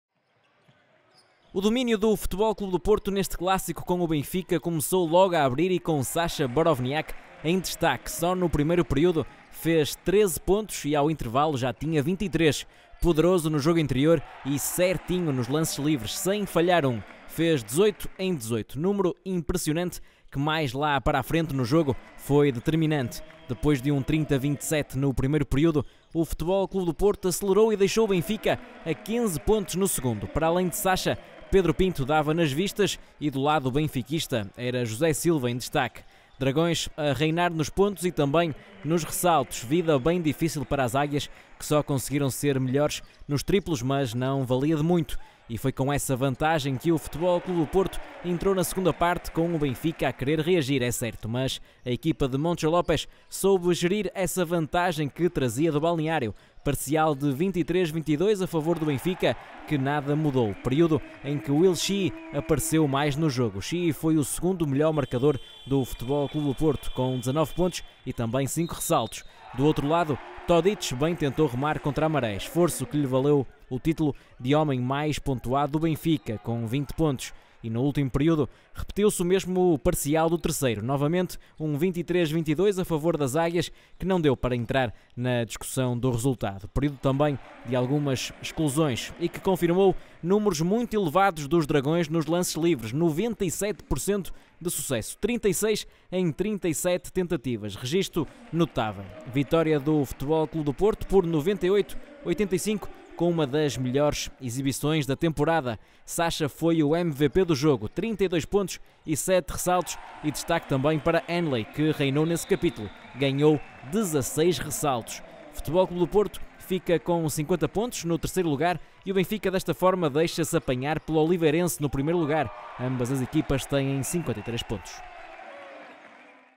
Basquetebol: FC Porto Benfica, 98-85, (Liga Portuguesa, 2.ª fase, 7.ª j., 22/04/18)
​O FC Porto recebeu e bateu este domingo o Benfica (98-85), no Dragão Caixa, em partida a contar para a sétima jornada da segunda fase da Liga Portuguesa de Basquetebol. Os azuis e brancos seguem na terceira posição do Grupo A, agora com 50 pontos, menos três do que Benfica e Oliveirense.